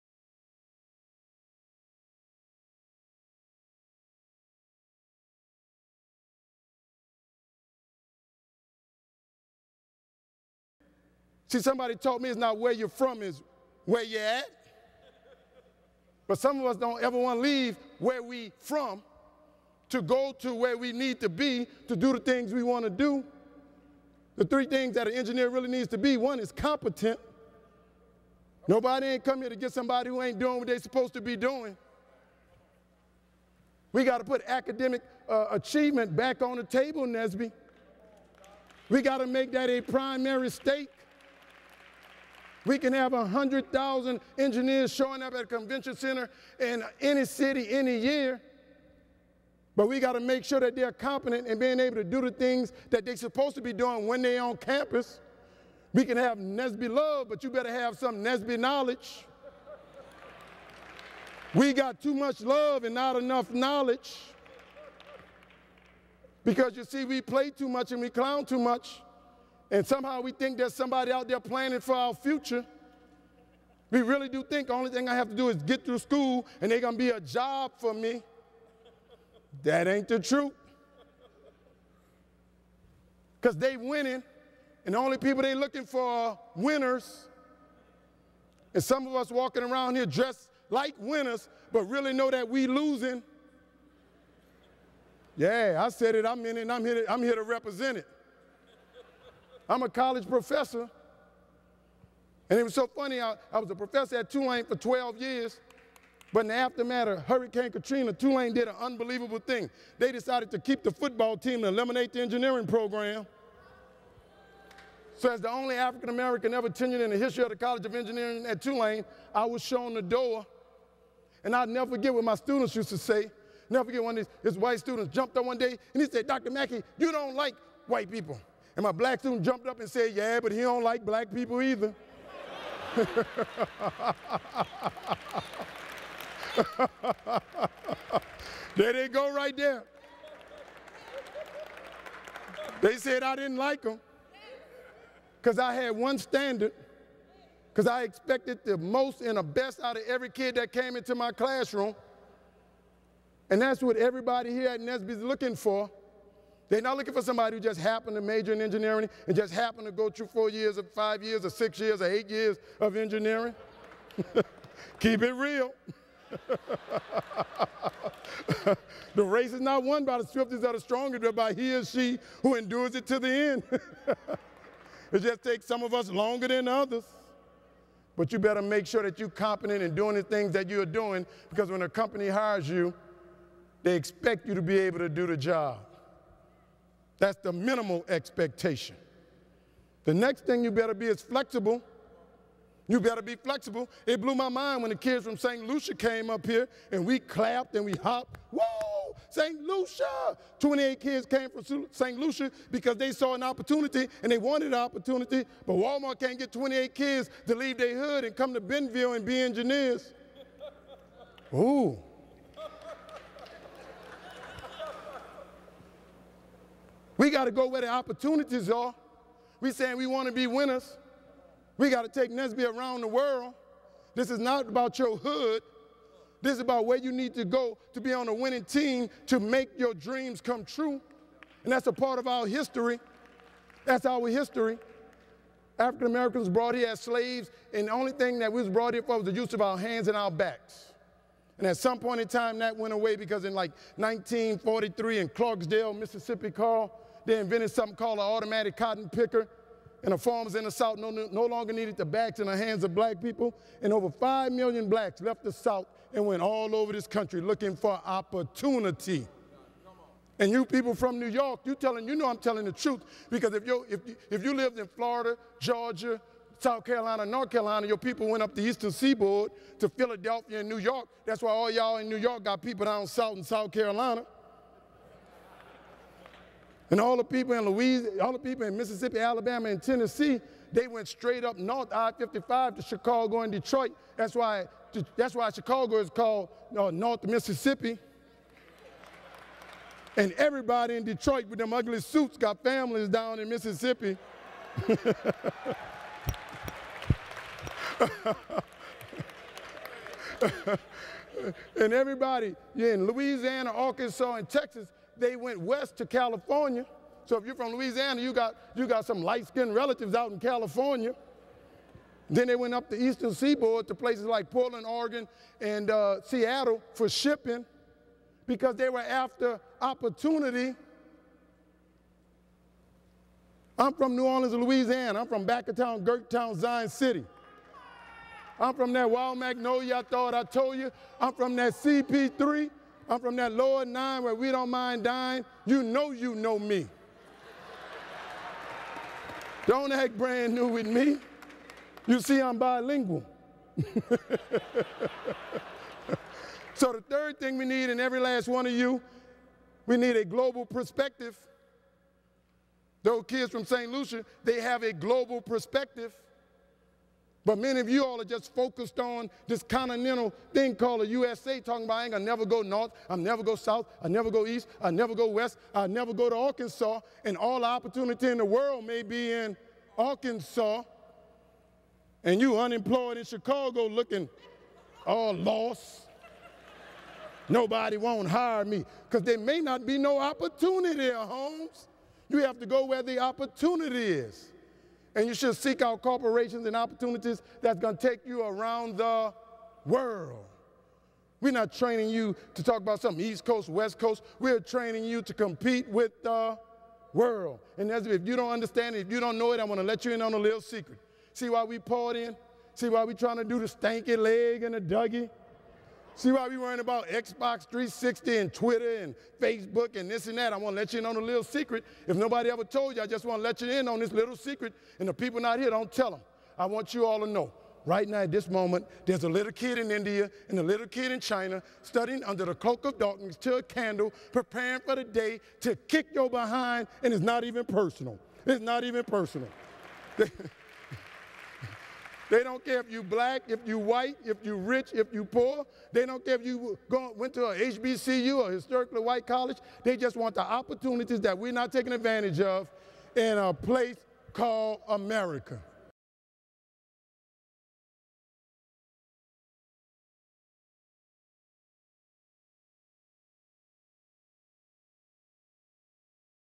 Dr. Calvin Mackie at NSBE - The Opportunity of Winning
Dr. Calvin Mackie speaks at the NSBE National Conference

Go Follow Dr. Calvin Mackie on Facebook, Twitter, & Instagram - @calvinmackie